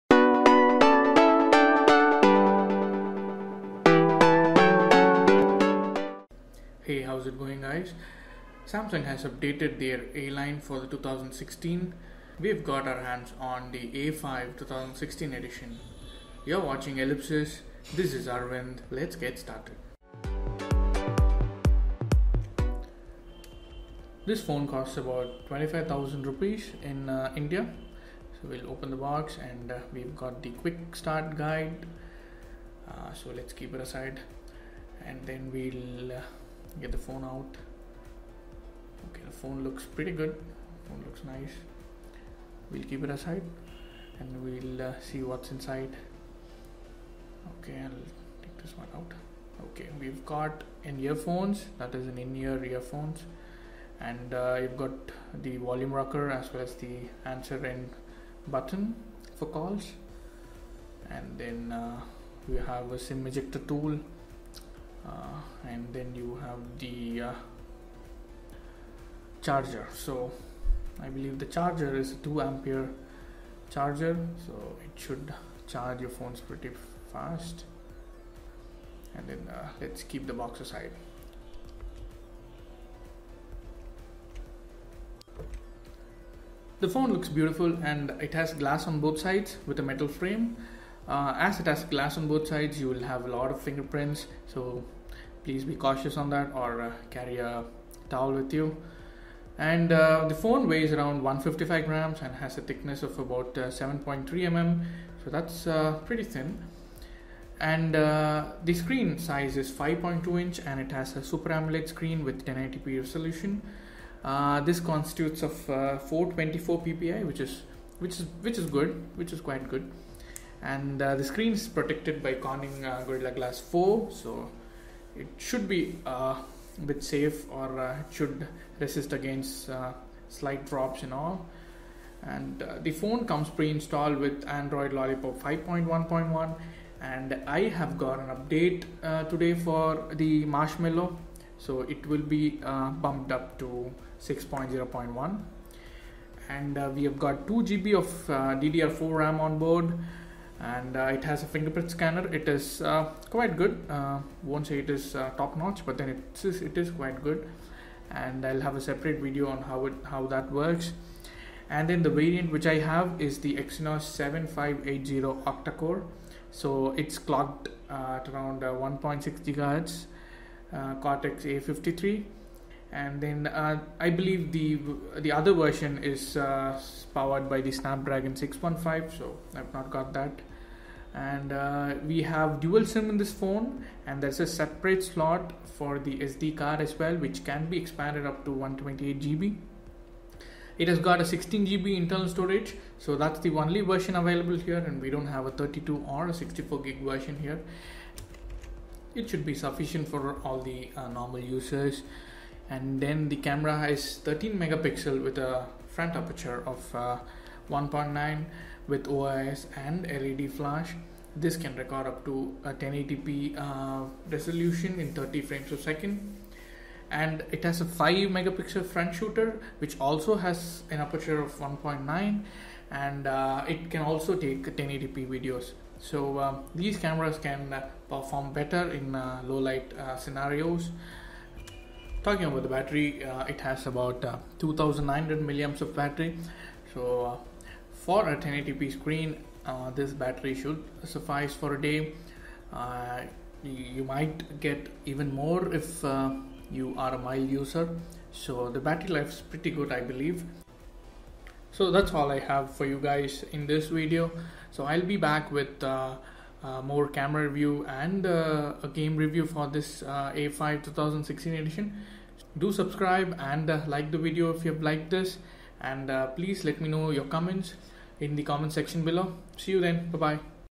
Samsung Galaxy A5 2016 - Unboxing and first impressions
Samsung Galaxy A5 2016 - Unboxing and first impressions. More In-depth analysis of the Camera and Performance is on the way!